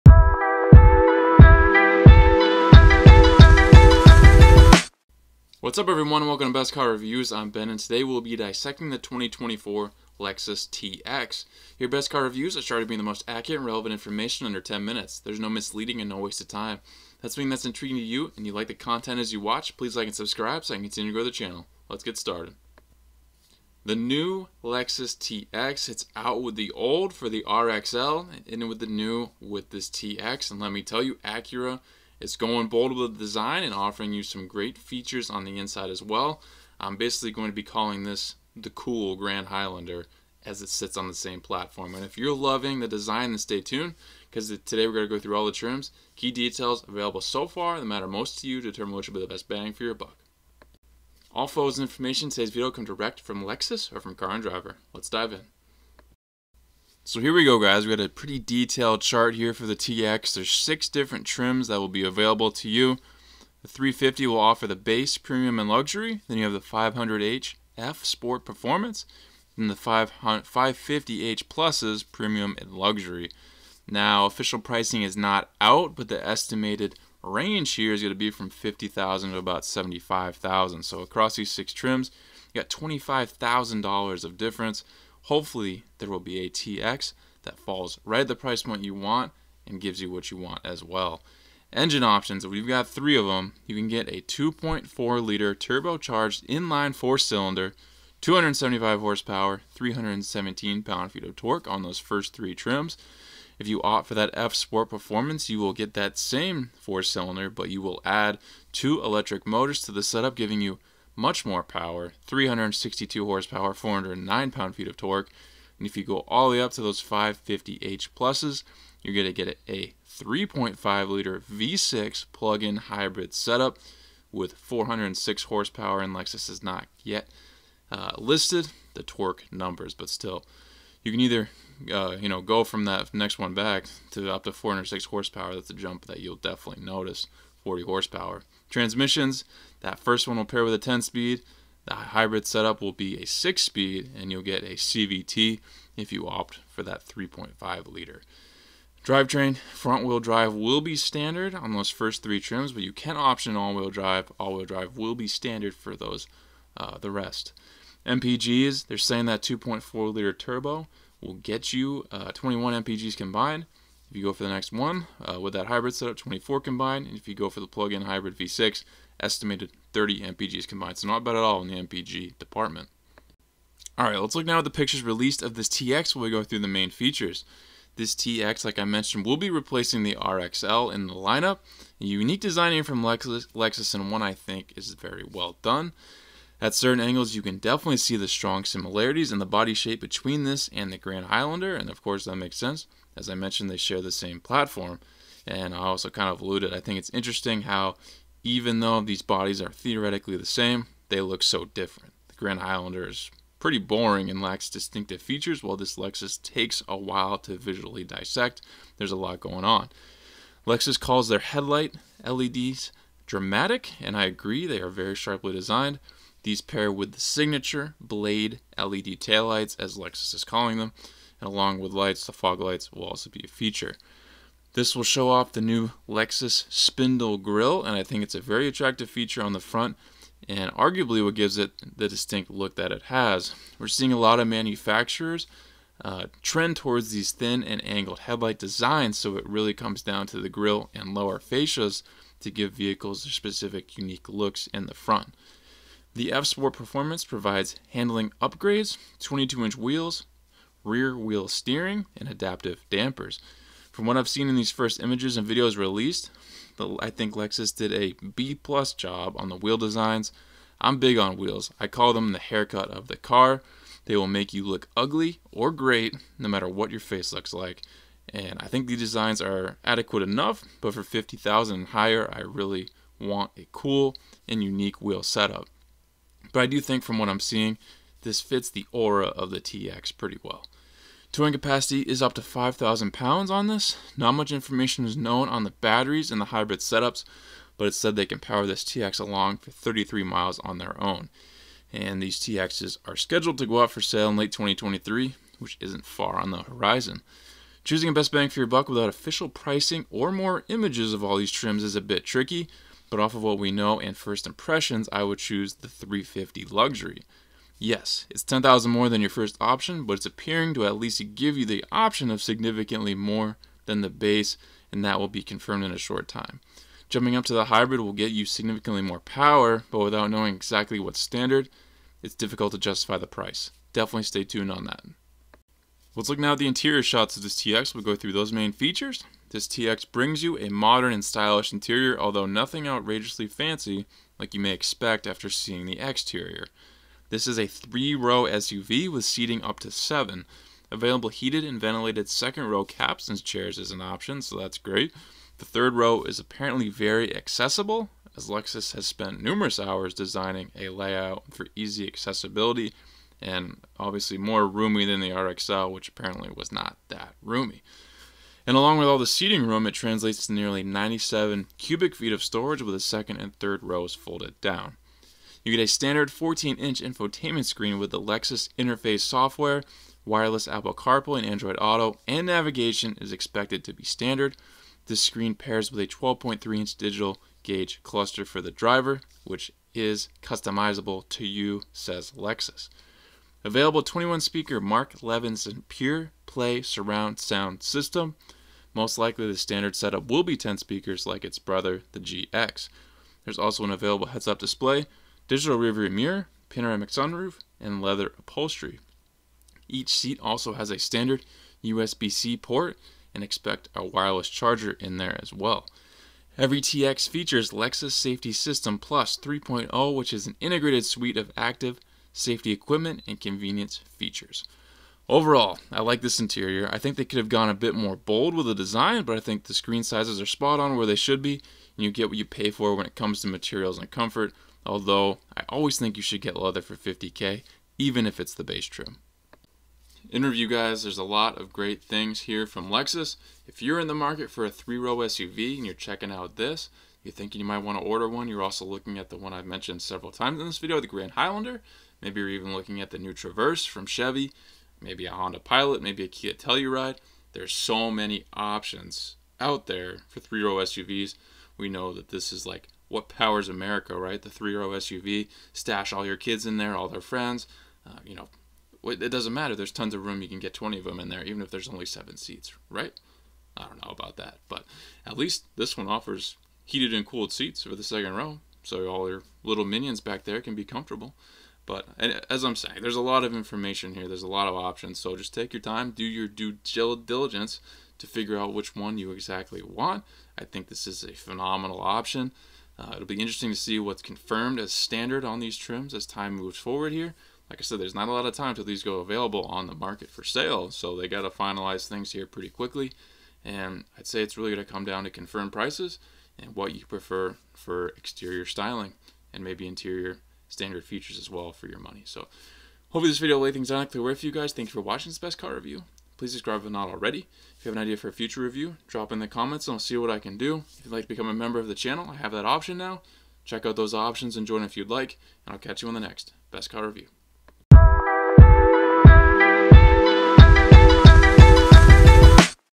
ALL NEW 2024 Lexus TX: Trims, Features & More!
Learn the trims and unique features of the brand new 2024 Lexus TX in this review! I compile the most relevant and important information released thus far. All photos and information direct from Lexus or Car & Driver.